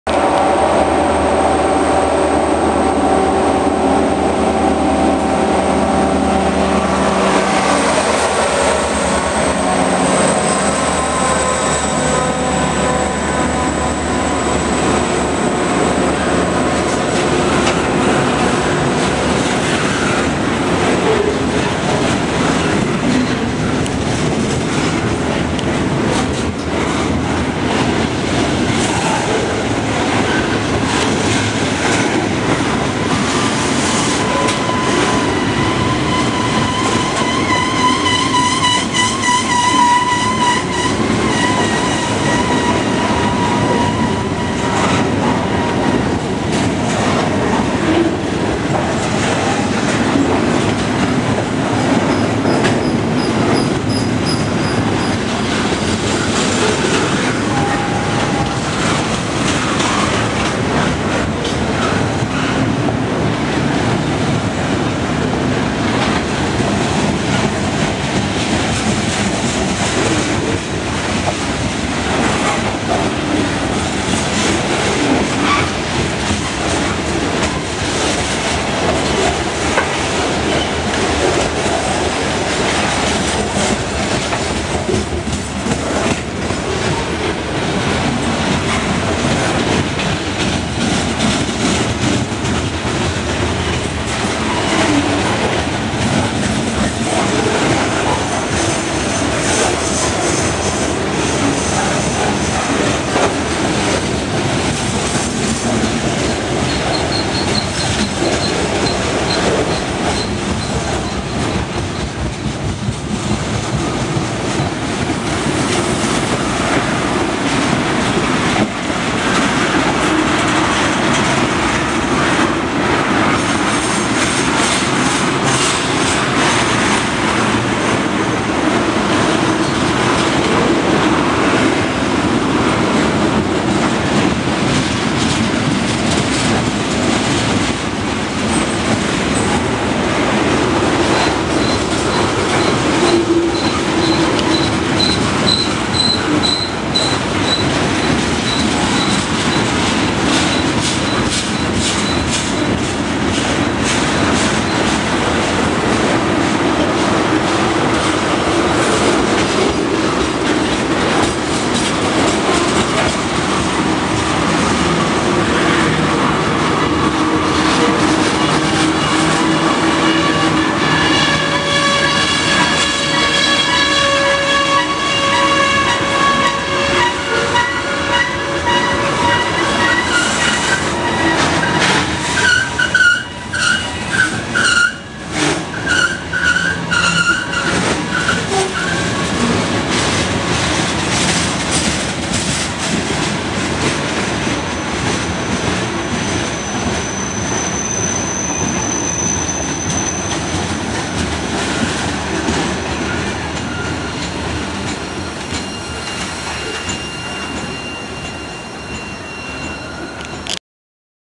Failed attempt to film NS K82
My camera was giving me issues, plus I had no time to get to a better spot. Oh well...